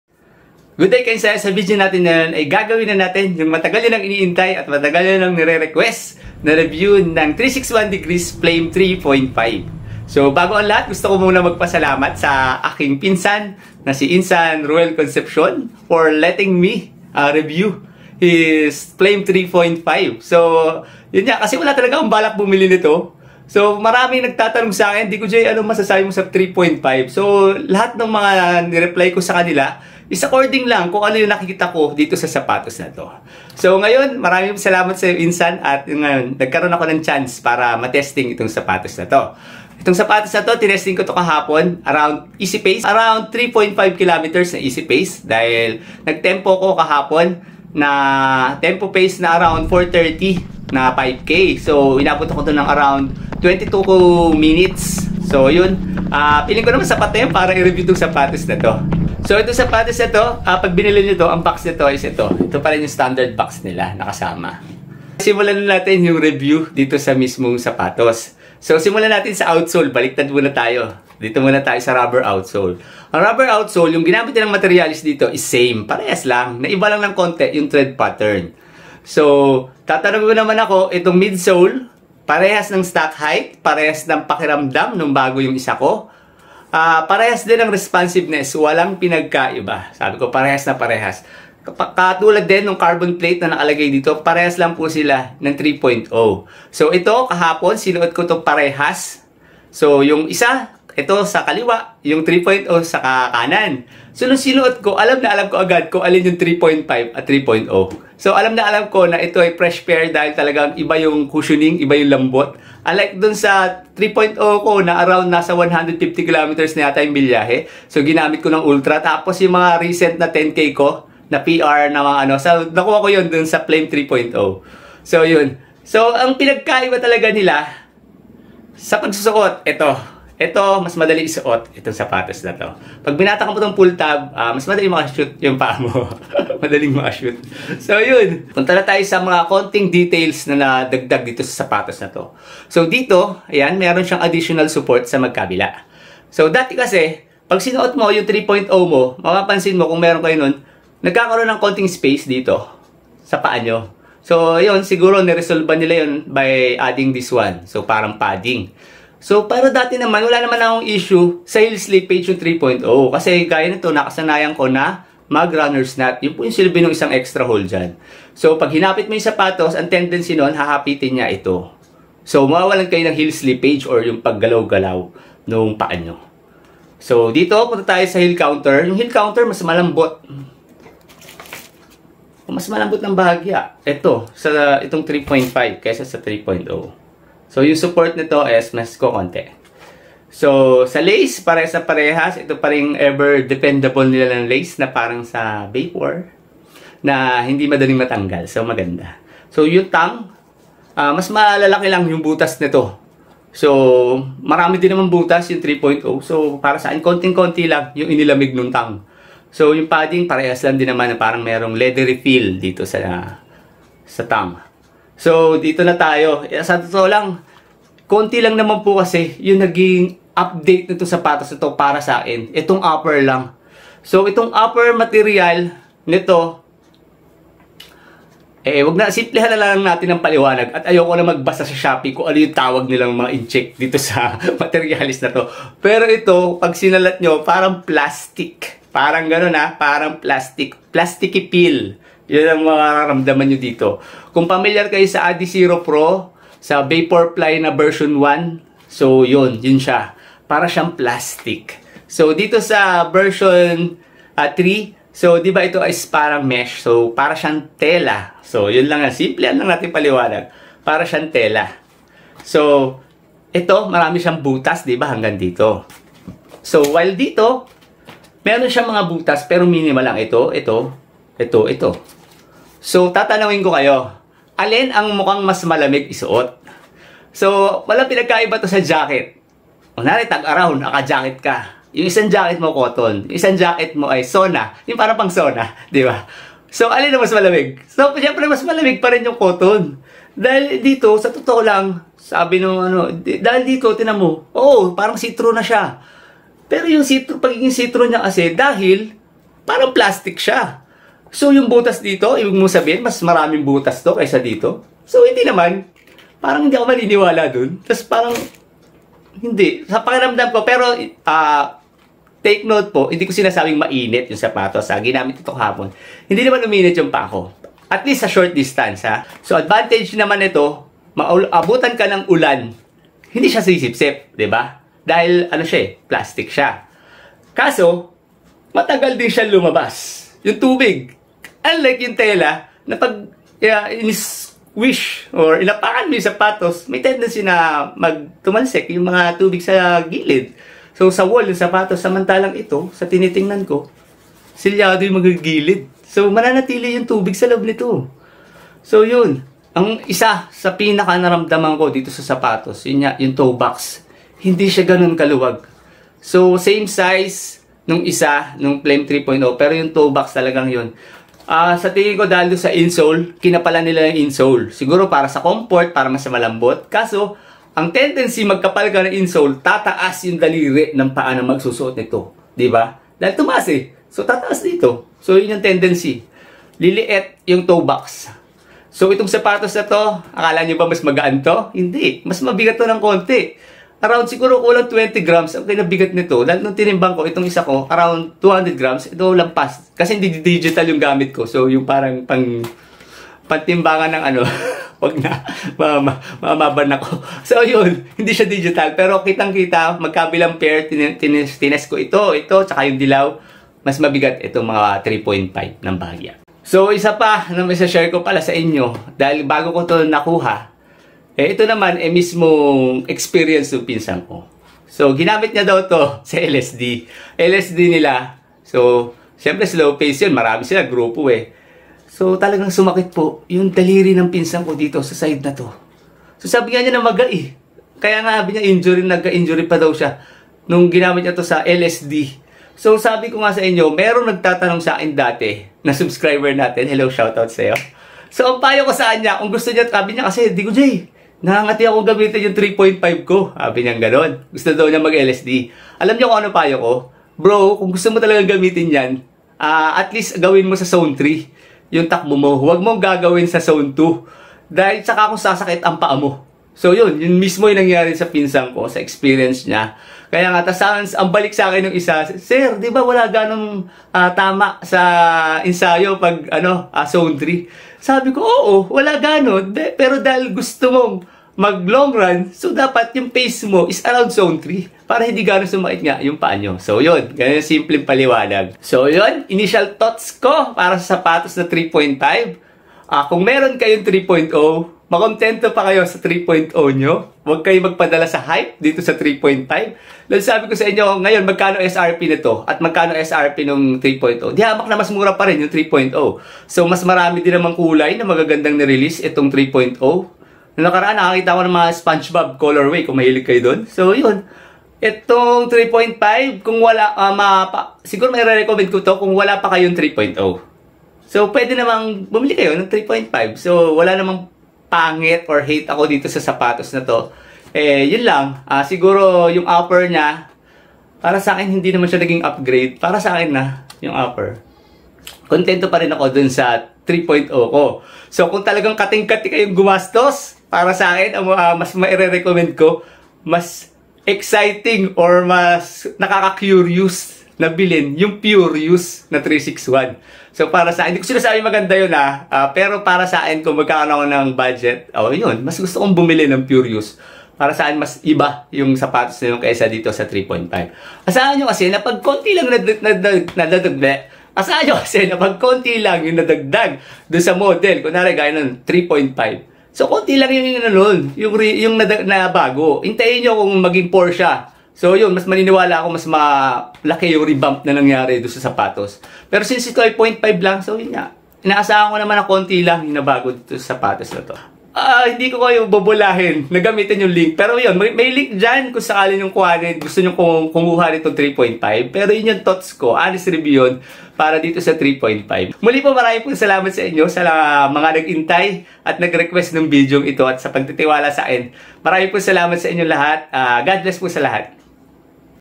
361 Degrees FLAME 3.5 REVIEW | UPGRADED ba or HYPE lang?
361 Degrees FLAME 3.5 REVIEW | UPGRADED ba or HYPE lang video Is my honest opinion about the shoe. As a fan of the flame 3 and still using it for my races, is the 3.5 a upgrade to consider?